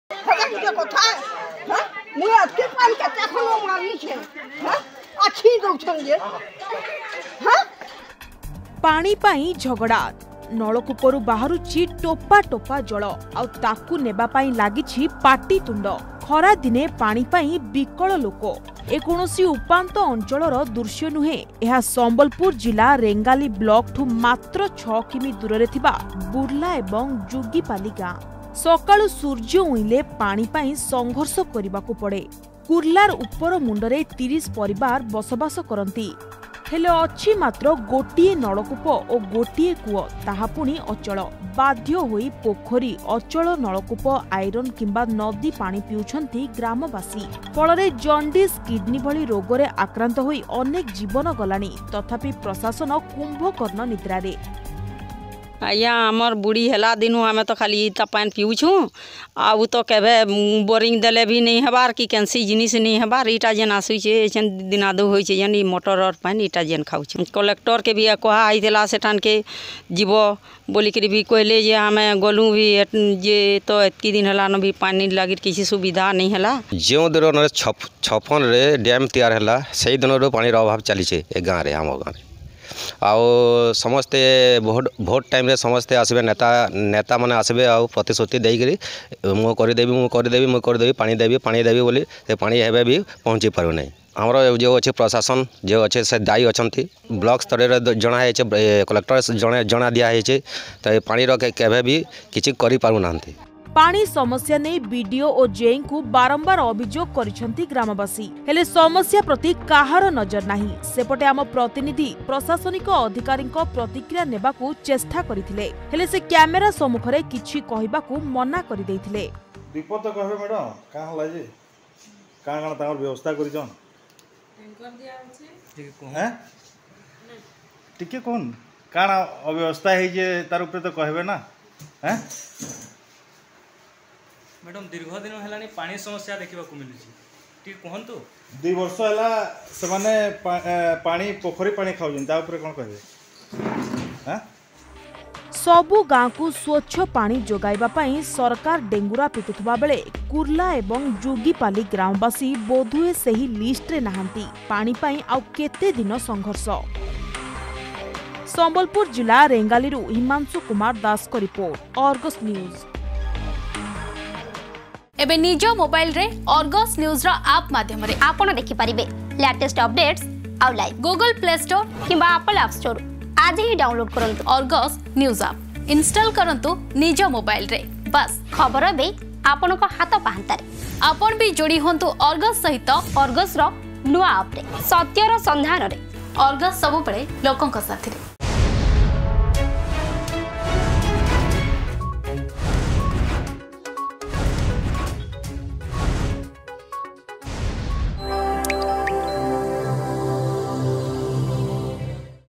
ଖରାଦିନେ ପାଣି ପାଇଁ କଲବଲ ...
ପାଣି ପାଇଁ ଝଗଡା... ନଳକୂପରୁ ବାହାରୁନି ଠୋପା ଠୋପା ଜଳ । ଆଉ ତାକୁ ନେବା ପାଇଁ ଲାଗିଛି ପାଟିତୁଣ୍ଡ । ଖରାଦିନେ ପାଣି ପାଇଁ ବିକଳ ଲୋକ । ଏ କୌଣସି ଉପାନ୍ତ ଅଞ୍ଚଳର ଦୃଶ୍ୟ ନୁହେଁ... ଏହା ସମ୍ବଲପୁର ଜିଲ୍ଲା ରେଙ୍ଗାଲି ବ୍ଲକଠୁ ମାତ୍ର 6 କିମି ଦୂରରେ ଥିବା କୁର୍ଲା ଏବଂ ଯୁଗୀପାଲି ଗାଁ...